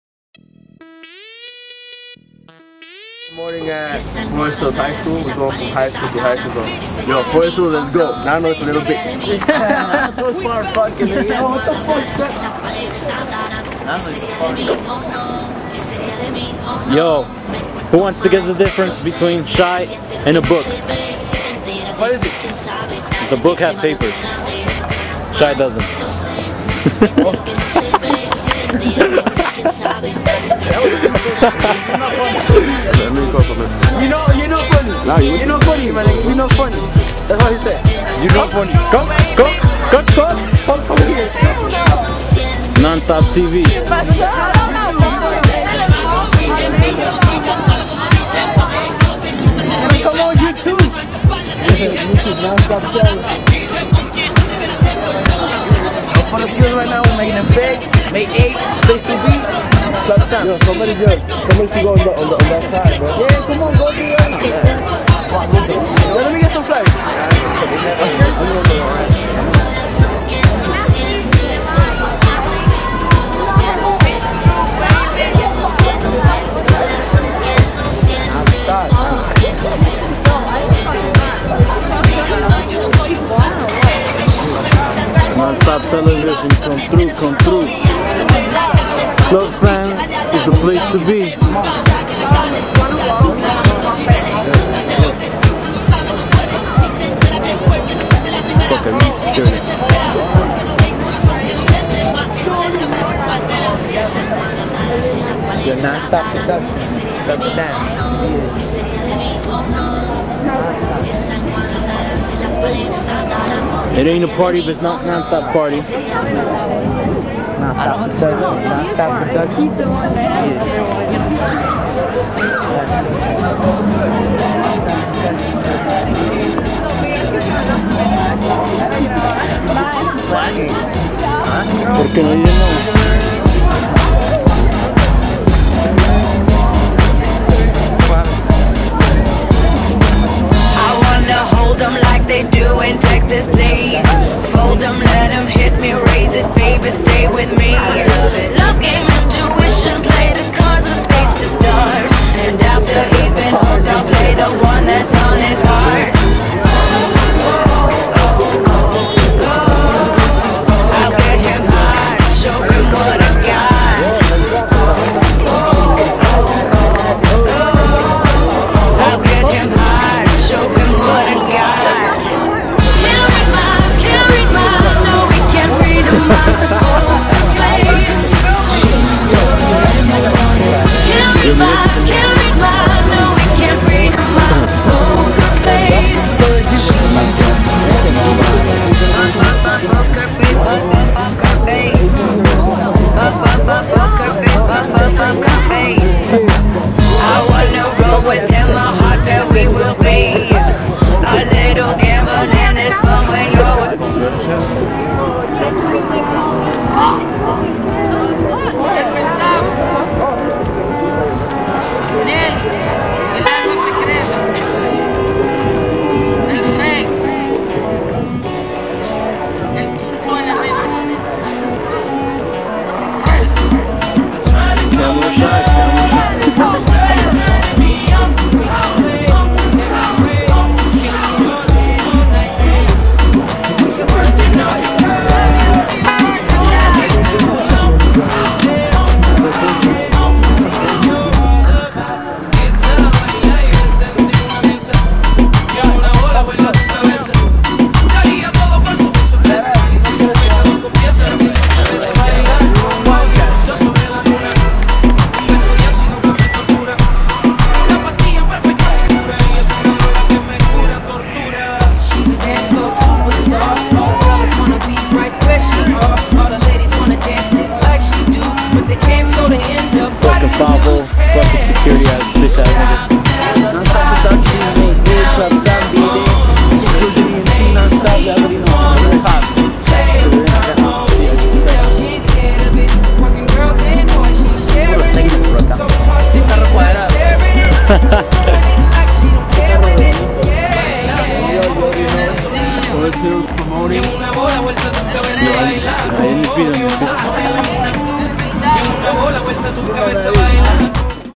Non Stop Promoting Teen Bash at Forest Hills High School May 8 at club Sanm Non Stop TV
SHOT &  EDITED BY NON STOP CHRIS
(NON STOP PRODUCTIONS)
LINKS:
MYSPACE.COM/NONSTOPCHRIS
FACEBOOK.COM/NONSTOPCHRIS
TWITTER.COM/NONSTOPCHRIS